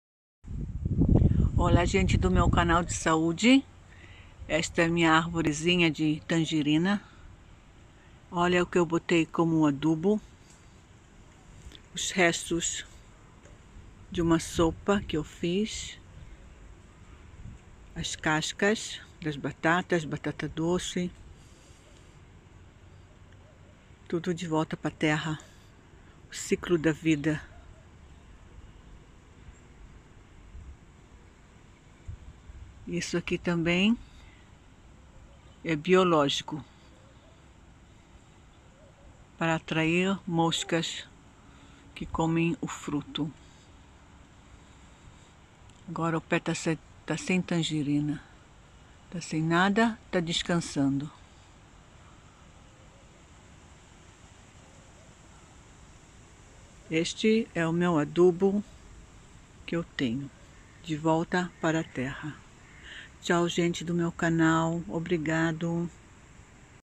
Saúde#ciclodavida#semaçúcar#semfarinhadetrigo#semtoxina#natureza#ecologica#longavida#semleite#
Moro em Israel, e sigo à natureza. A saúde está na natureza. As letras D e u s estão dentro da palavra Saúde. Viram? Podem conferir. Os remédios estão na natureza. Comer natural. Comer fresco. Halina de Israel.